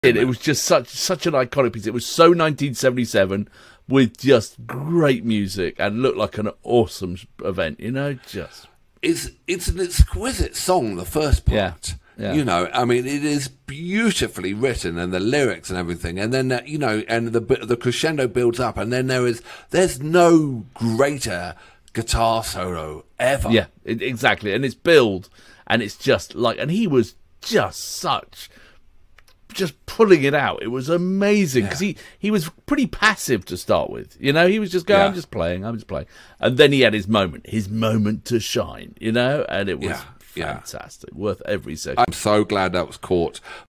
The World's most Iconic Guitar Solo - Lynyrd Skynyrd
Can Lynyrd Skynyrd deliver the worlds most iconic guitar solo ever?